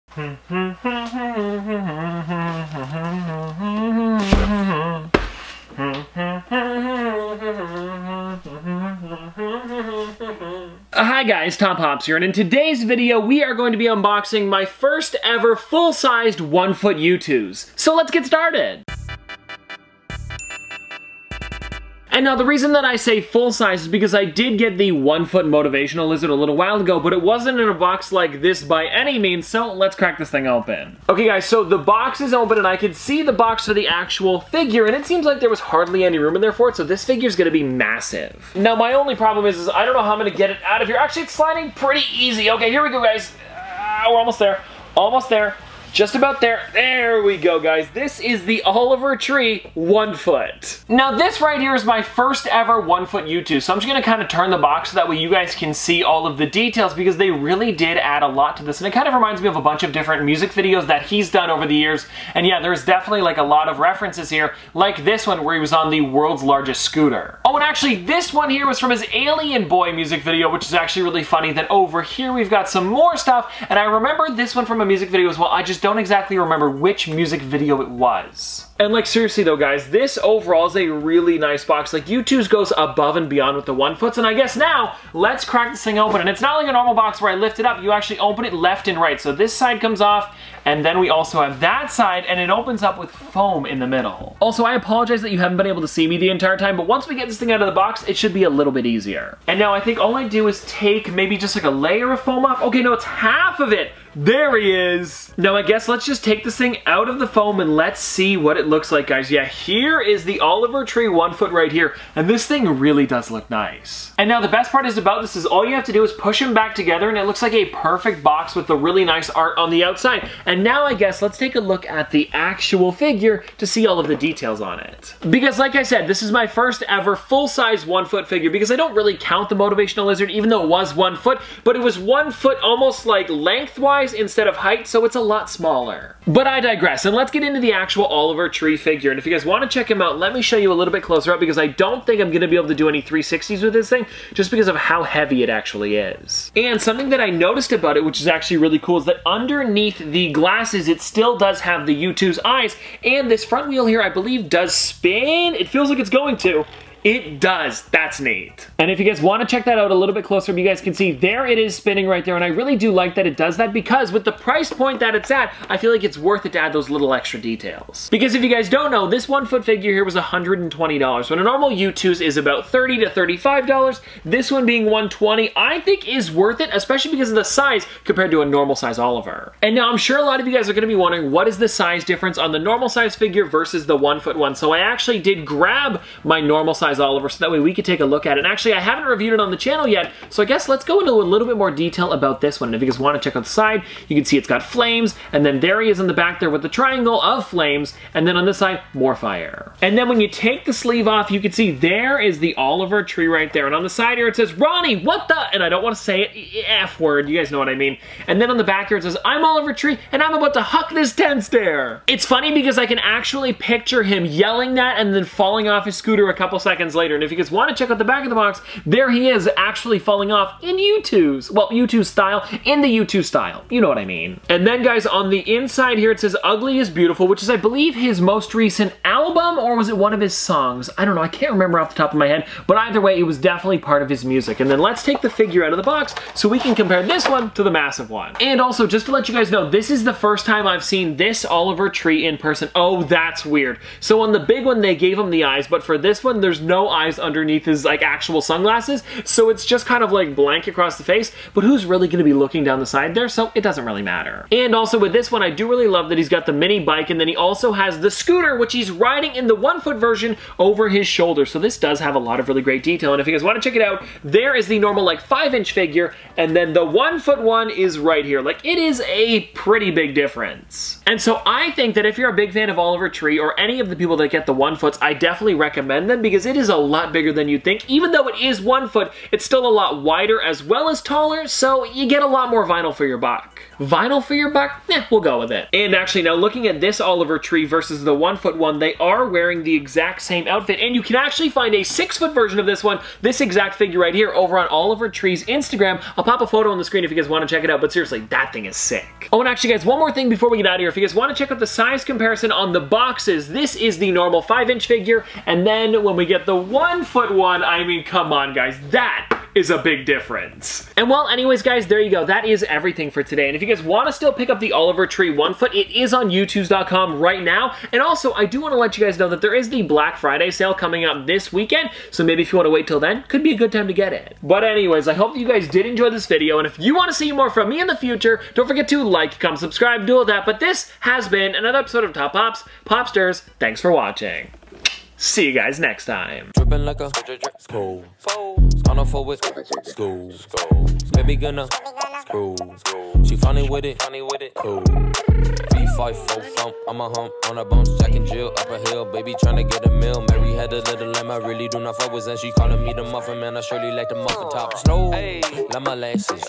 My Biggest Youtooz Package Yet!
Today we are unboxing my first 1ft Youtooz!